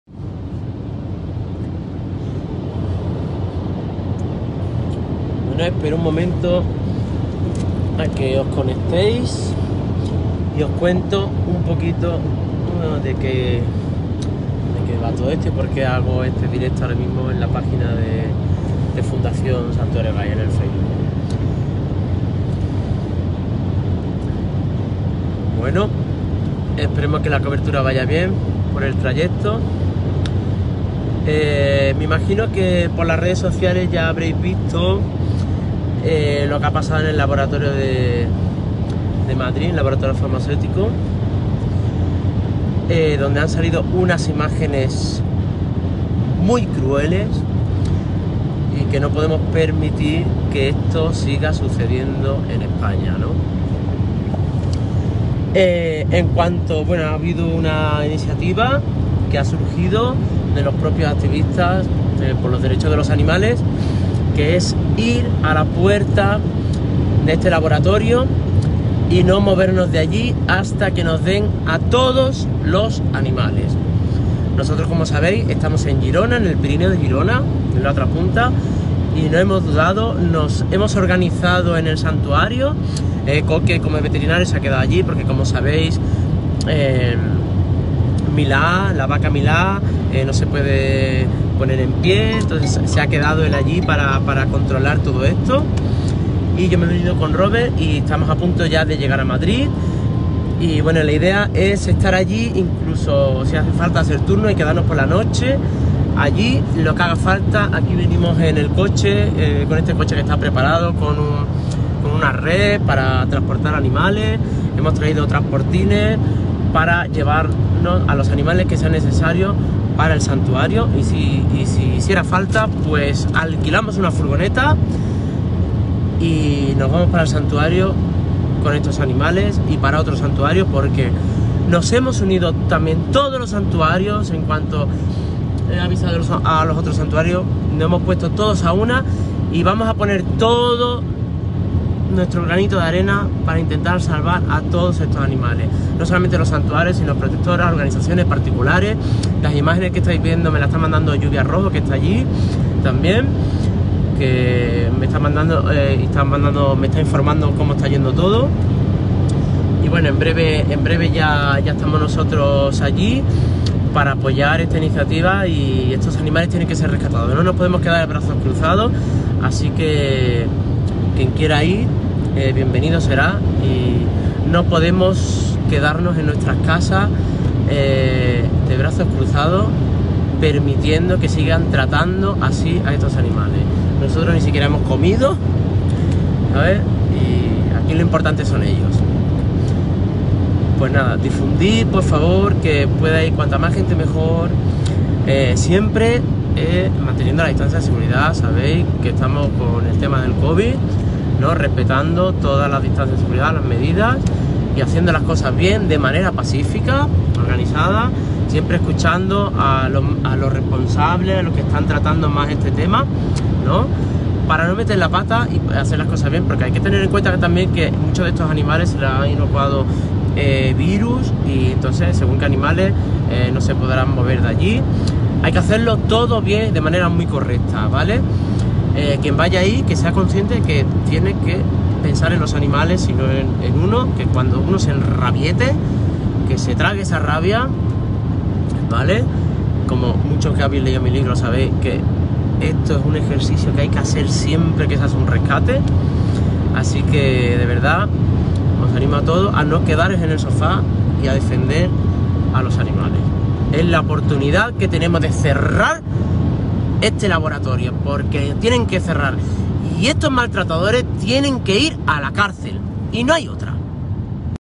Laboratorio Vivotecnia
Nos vemos en QUBE Technology Park

Tenemos que cerrar el laboratorio farmacéutico de Madrid Vivotecnia y rescatar a todos los animales.